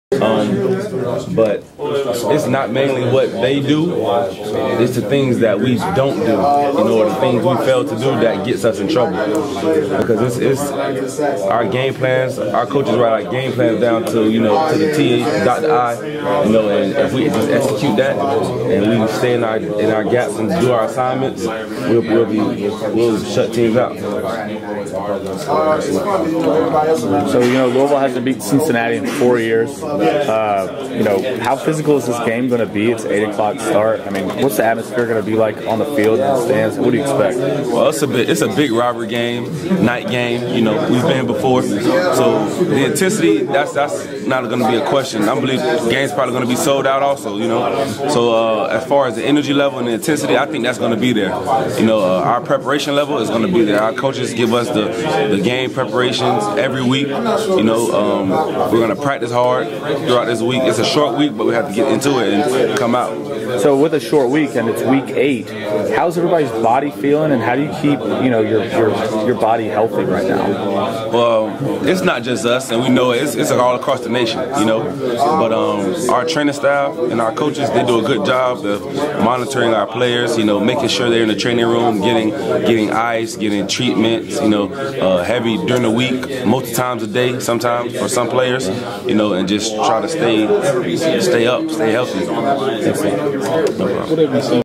George Durant Cincinnati Pre-Game 2012
Louisville Linebacker George Durant talks about his team prior to his #14 ranked Louisville Cardinals face the Cincinnati Bearcats on Friday night on ESPN